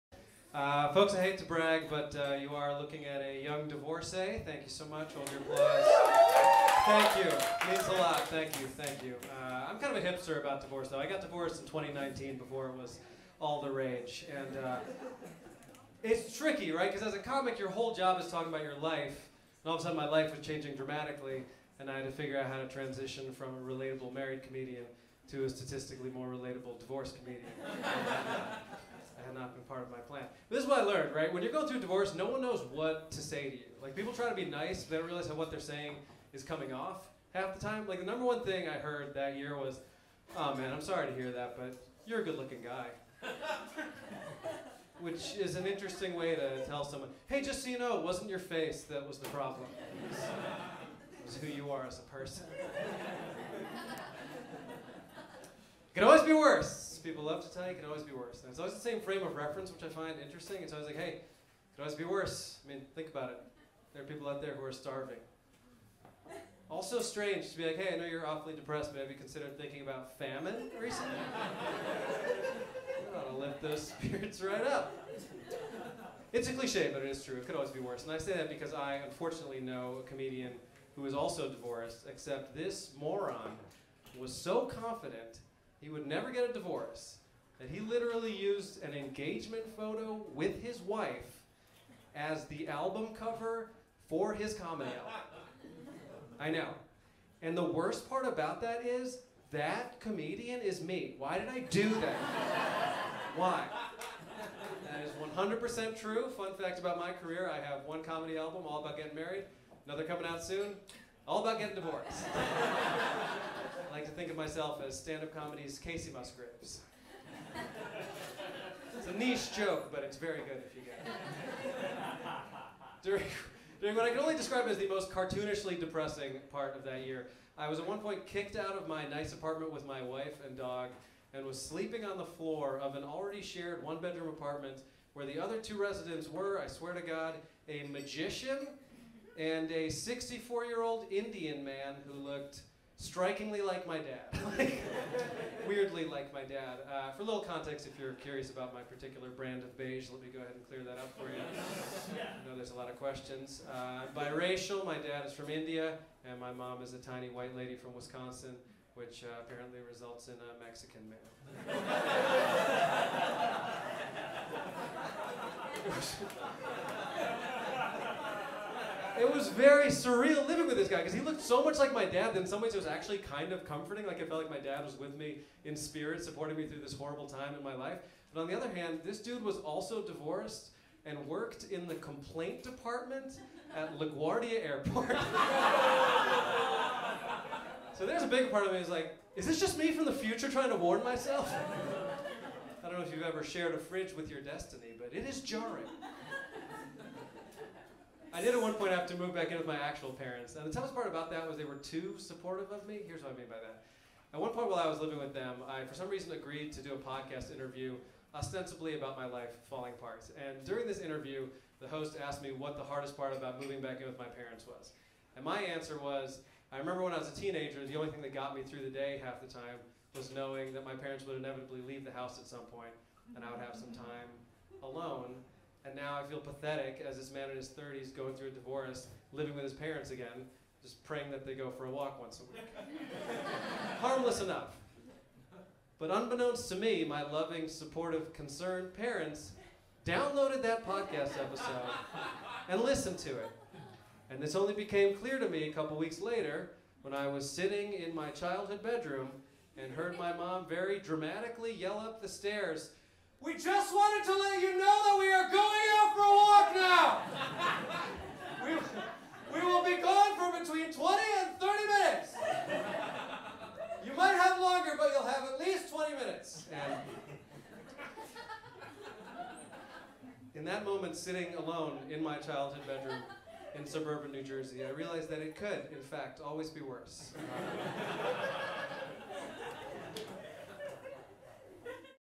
Adam Mamawala Comedians You Should Know NYC 4/6/22
Some old stuff, some new stuff. Hope ya dig it. New album out soon!

Set from CYSK NYC at The Gutter in Brooklyn, NY.